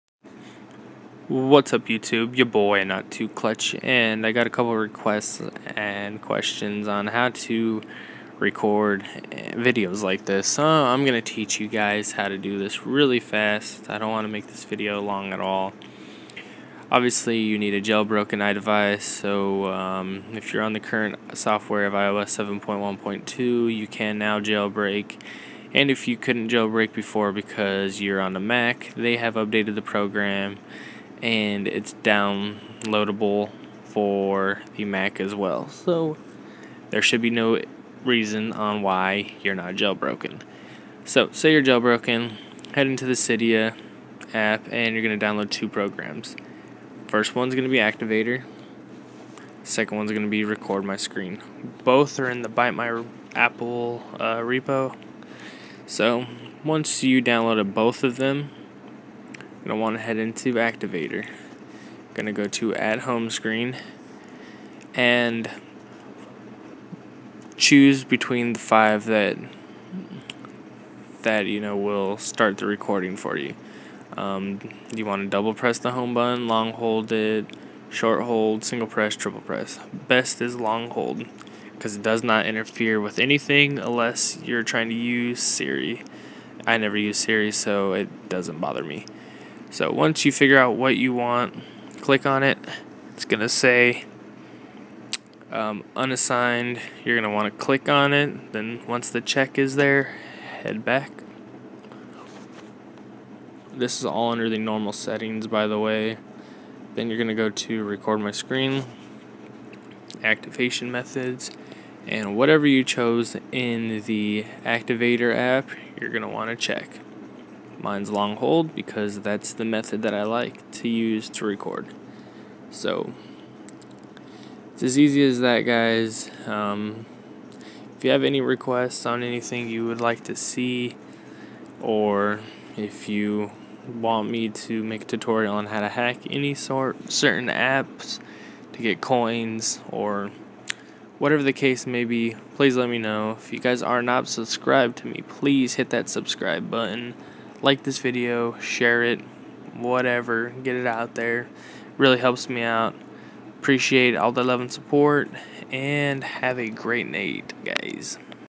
How to record iphone screen ios 7.1.2
This video will teach you how to record your screen on your idivice running ios 7.1.2